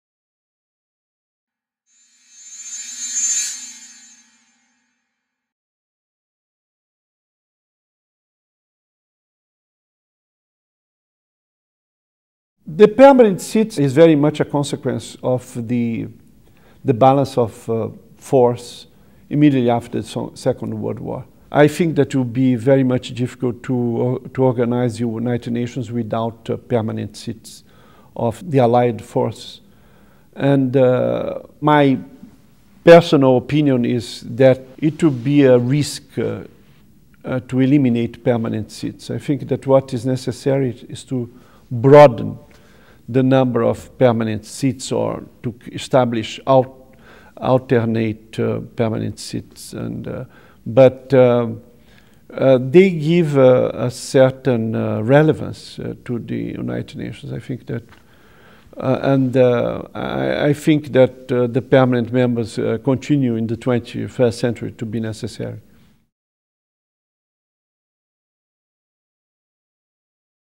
Why are there permanent seats in the UN Security Council?
Paulo Sérgio Pinheiro, Brown University.
This video is part of the following Choices Program curriculum unit: The United Nations: Challenges and Change